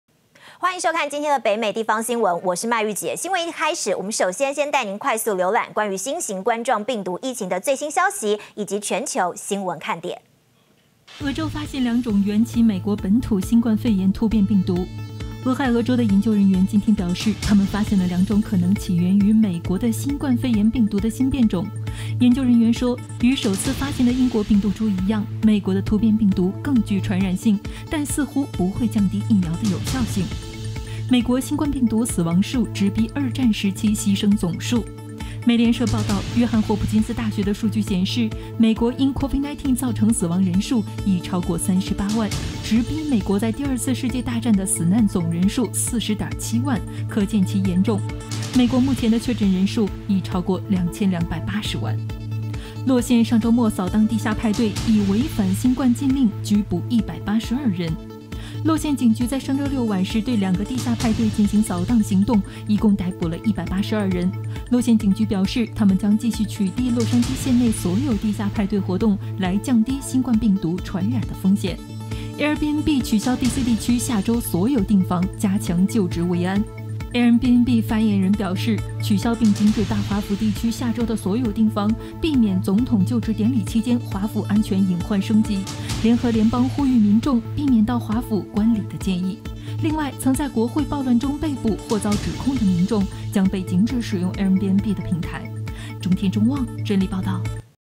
2021.01.13中天北美新聞-1 COVID-19 全球疫情實時更新
新聞的一開始，首先帶您快速瀏覽關於新型冠狀病毒疫情的最新消息，以及全球新聞看點。